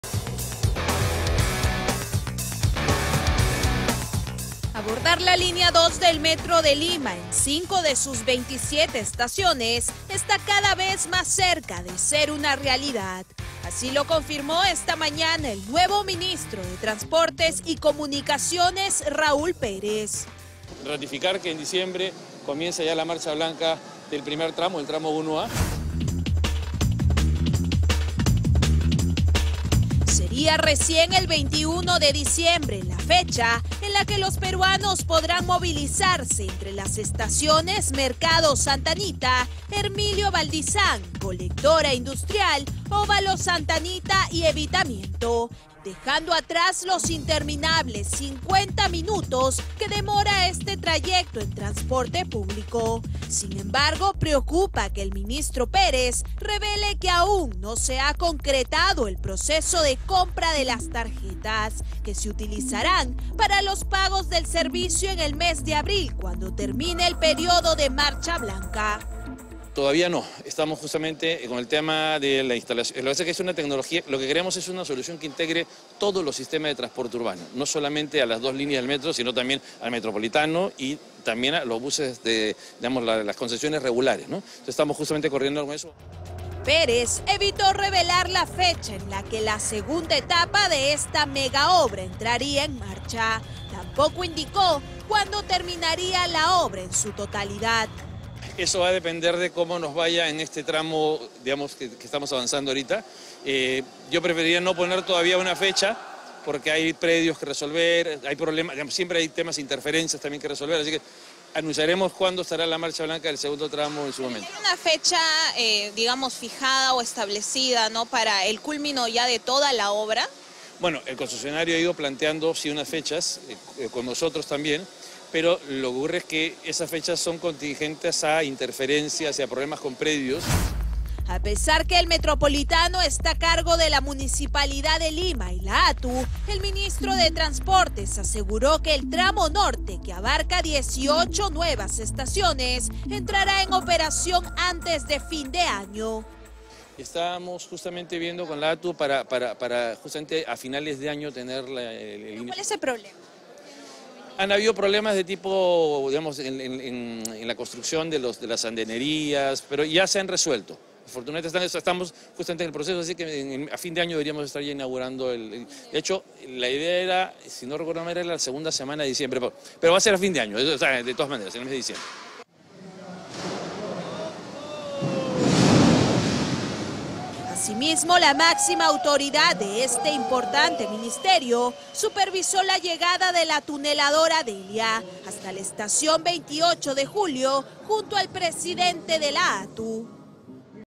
🚇🌟 ¡Línea 2 del Metro de Lima cada vez más cerca de ser una realidad! 🌟🚇
La Línea 2 del Metro de Lima, que comprende 27 estaciones, está cada vez más cerca de ser una realidad. Así lo confirmó el ministro de Transportes y Comunicaciones, Raúl Pérez, durante una supervisión de la obra.

A pesar de que el Metropolitano está a cargo de la Municipalidad de Lim​a y la Autoridad de Transporte Urbano (ATU), el Ministro de Transportes aseguró que el tramo norte, que comprende 18 nuevas estaciones, entrará en operación antes de que termine el año.

Durante su visita, la máxima autoridad de este importante ministerio supervisó la llegada de la tuneladora Delia hasta la estación 28 de julio, en compañía del presidente de la ATU. Sin embargo, hay ciertas preocupaciones que aún persisten.

Una de ellas es que el Ministro Pérez reveló que aún no se ha concretado el proceso de compra de las tarjetas que se utilizarán para los pagos del servicio en el mes de abril, cuando termine el periodo de marcha blanca. Además, Pérez evitó revelar la fecha en la que la segunda etapa de esta mega obra entraría en marcha. Tampoco indicó cuándo terminaría la obra en su totalidad.

CRONOGRAMA

Según el cronograma actual, los peruanos podrán movilizarse entre las estaciones Mercado Santa Anita, Hermilio Valdizán, Colectora Industrial, Óvalo Santa Anita y Evitamiento en solo 15 minutos, a partir del 21 de diciembre.